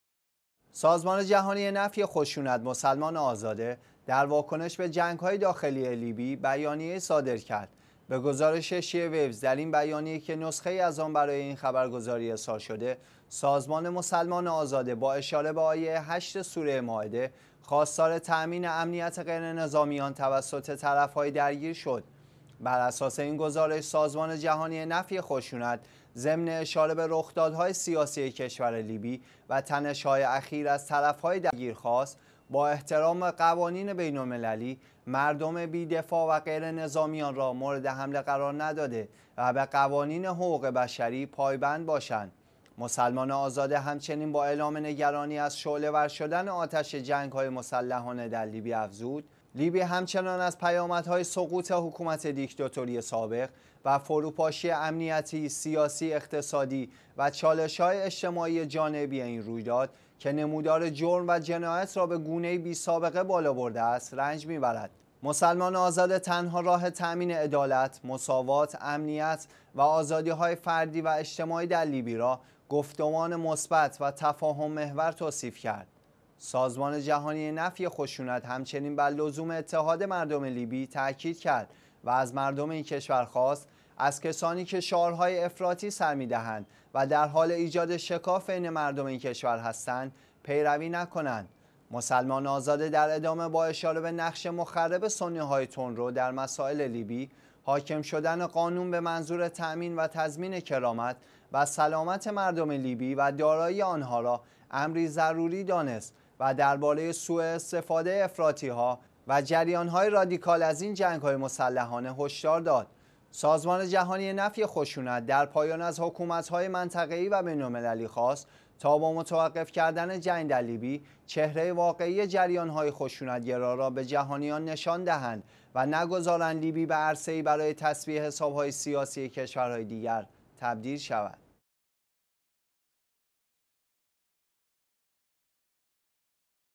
تاکید سازمان جهانی نفی خشونت بر تامین امنیت غیر نظامیان در لیبی
سازمان جهانی نفی خشونت ـ مسلمان آزاده ـ در واکنش به جنگ های داخلی لیبی، بیانیه ای صادر کرد.
به گزارش شیعه ویوز، در این بیانیه که نسخه ای از آن برای این خبرگزاری ارسال شده، سازمان مسلمان آزاده با اشاره به آیه 8 سوره مائده، خواستار تامین امنیت غیر نظامیان توسط طرف های درگیر شد.
بر اساس این گزارش، سازمان جهانی نفی خشونت ضمن اشاره به رخدادهای سیاسی کشور لیبی و تنش های اخیر،  از طرف های درگیر خواست با احترام به قوانین بین المللی، مردم بی دفاع و غیر نظامیان را مورد حمله قرار نداده و به قوانین حقوق بشری پای بند باشند.
مسلمان آزاده همچنین با اعلام نگرانی از شعله ور شدن آتش جنگ های مسلحانه در لیبی، افزود: لیبی همچنان از پیامدهای سقوط حکومت دیکتاتوری سابق و فروپاشی امنیتی، سیاسی، اقتصادی و چالش های اجتماعیِ جانبی این رویداد که نمودار جرم و جنایت را به گونه ای بی سابقه بالا برده است، رنج می برد.
مسلمان آزاده  تنها راه تامین عدالت، مساوات، امنیت و آزادی های فردی و اجتماعی در لیبی را  گفتمان مثبت و تفاهم محور توصیف کرد.
سازمان جهانی نفی خشونت، همچنین برلزوم اتحاد مردم لیبی تاکید کرد و از مردم این کشور خواست از کسانی که شعارهای افراطی سرمی دهند و در حال ایجاد شکاف بین مردم این کشور هستند، پیروی نکنند.
مسلمان آزاده در ادامه با اشاره به نقش مخرب سنی های تندرو در مسائل لیبی، حاکم شدن قانون به منظور تامین و تضمین کرامت و سلامت مردم لیبی و دارایی آن ها را،  امری ضروری دانست و در باره ی سوءاستفاده افراطی ها و جریان های رادیکال  از این جنگ های مسلحانه،  هشدار داد.
سازمان جهانی نفی خشونت(مسلمان آزاده)، در پایان از حکومت های منطقه ای و بین المللی خواست تا با متوقف کردن جنگ در لیبی، چهره واقعی جریان های خشونت گرا را به جهانیان نشان دهند، و نگذارند لیبی به عرصه ای برای تسویه حساب های سیاسی کشورهای دیگر تبدیل شود.